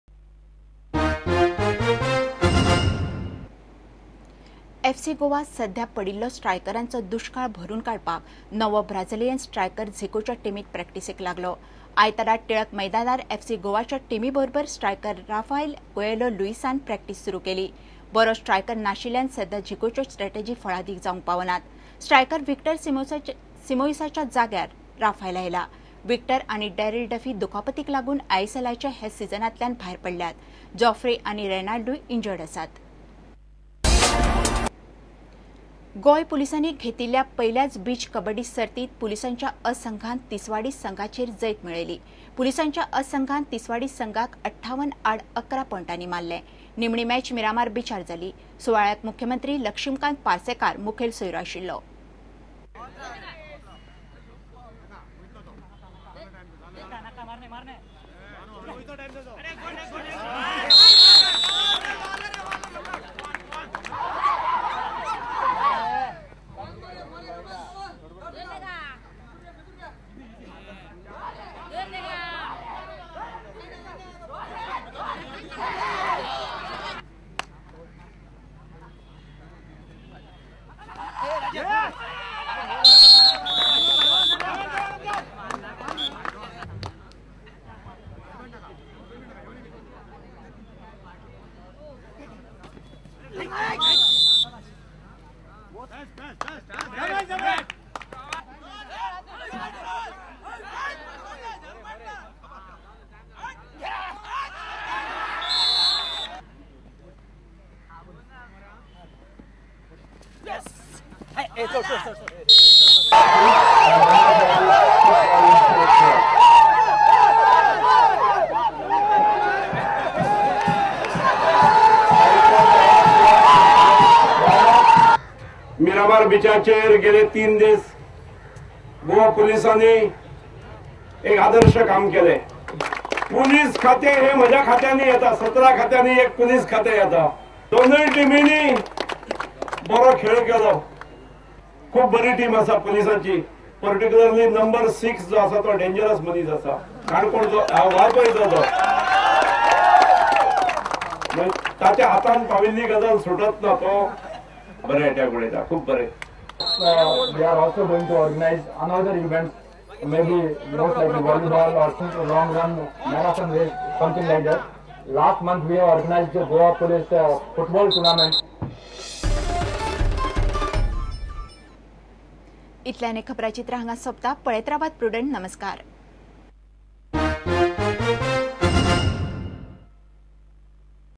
Prudent Media Konkani Prime News 01 Nov 15 Part 3
FC Goa's new entrant Brazilian striker Rafael Coleho begins training at Tilak maidan,  Rafael replaces Victor Simoes, AIFF yet to clear Rafael for FC Goa